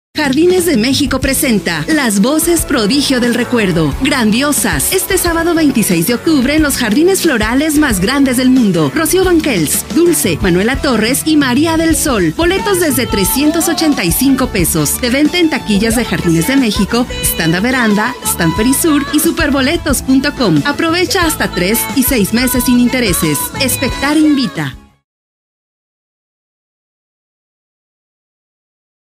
GranDiosas
Los Jardines florales más grandes del mundo te invita a que disfrutes de ManoellaTorres RocioBanquells Dulce MaríadelSol, adquiriendo tus boletos desde $385 pesos a 3 y 6 meses con tarjetas participantes, no te pierdas de una noche mágica en compañía de GranDiosas este 26 de octubre en JardinesDeMéxico. o en taquillas, stands de Averanda y Perisur en CDMX